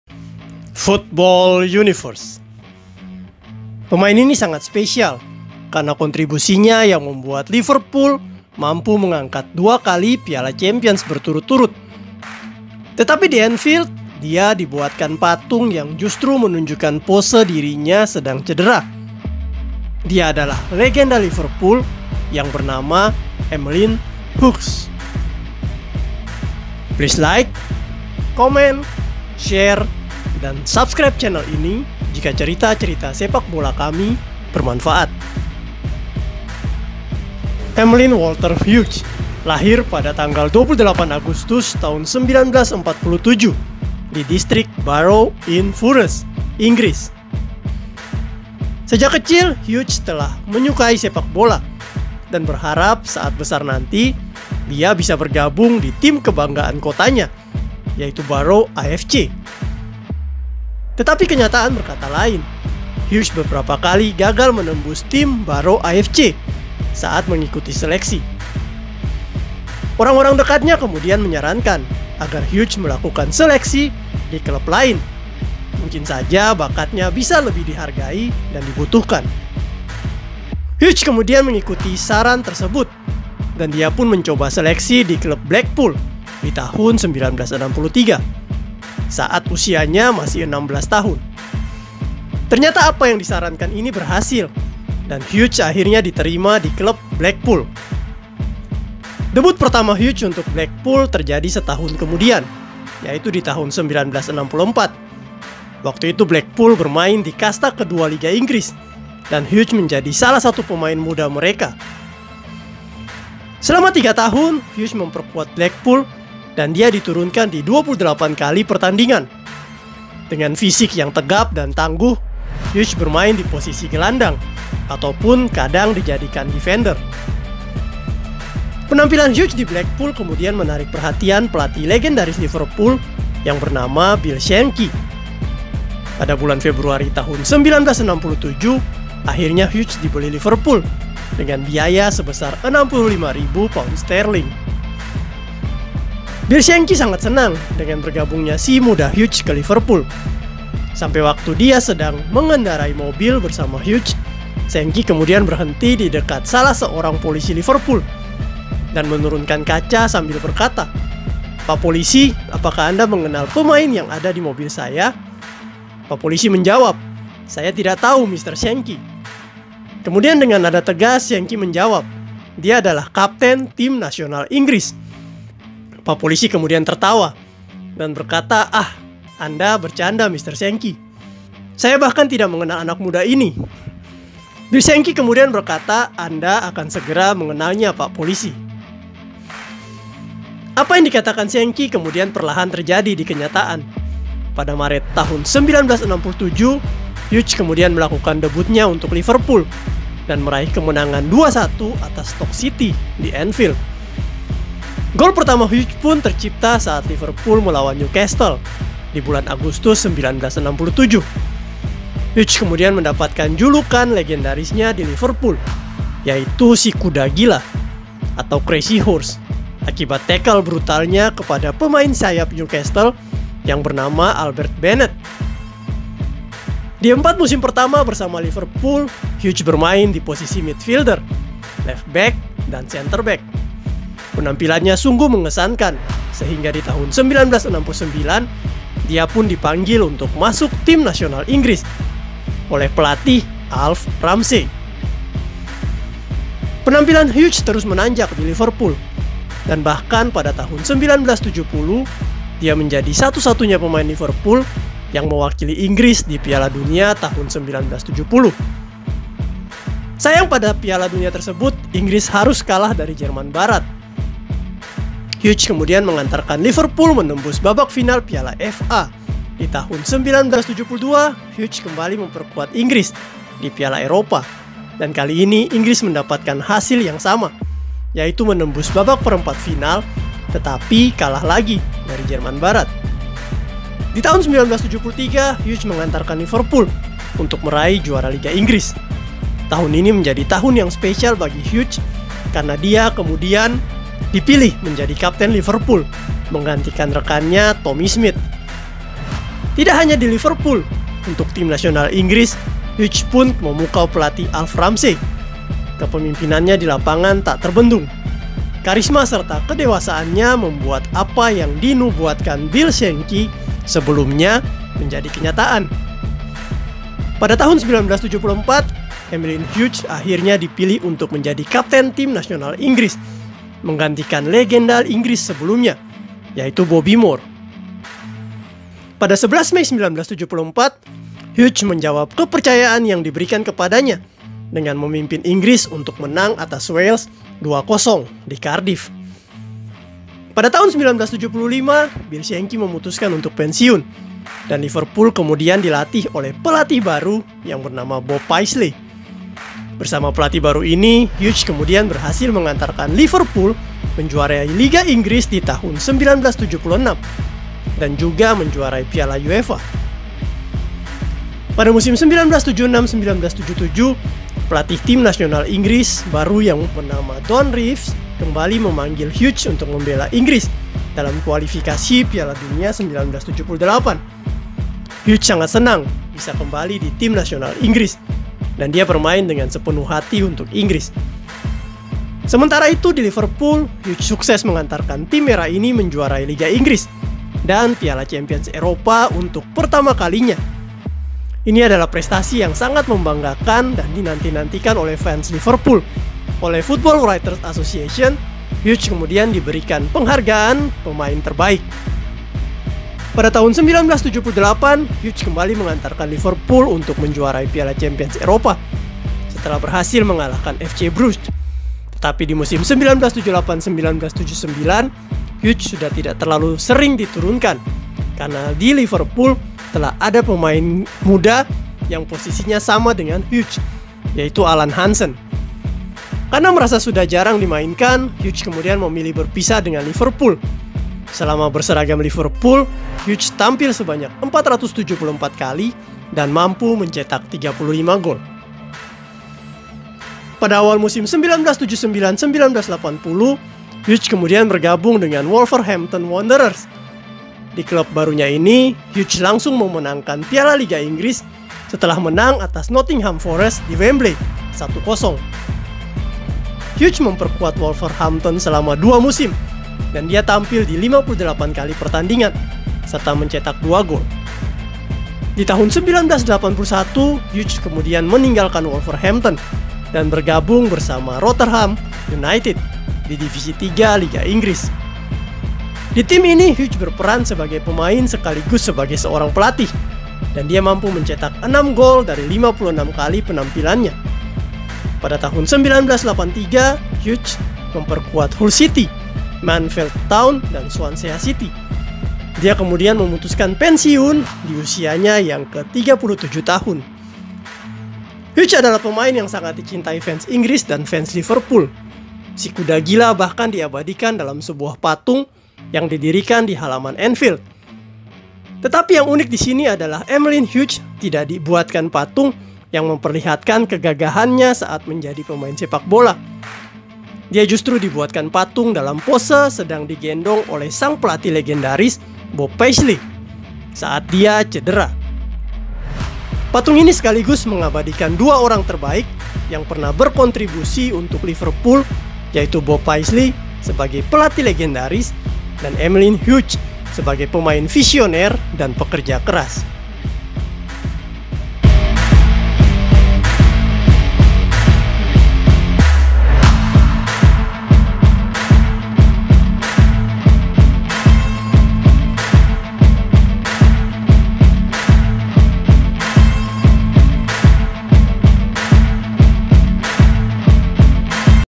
Emlyn Hughes Si Kuda Gila Yang Dibuatkan Patung Sedang Cedera (Legenda Liverpool & Inggris)
Legenda Liverpool & Inggris ini dijuluki si kuda gila. Permainannya selalu memukau dan di barisan pertahanan dia selalu kokoh. Dia berhasil mengantarkan Liverpool dua kali juara Piala Champions dan juga juara liga Inggris. Di Halaman Anfield dia dibuatkan patung bersama pelatih legendaris Bob Paisley, tetapi dalam kondisi sedang digotong.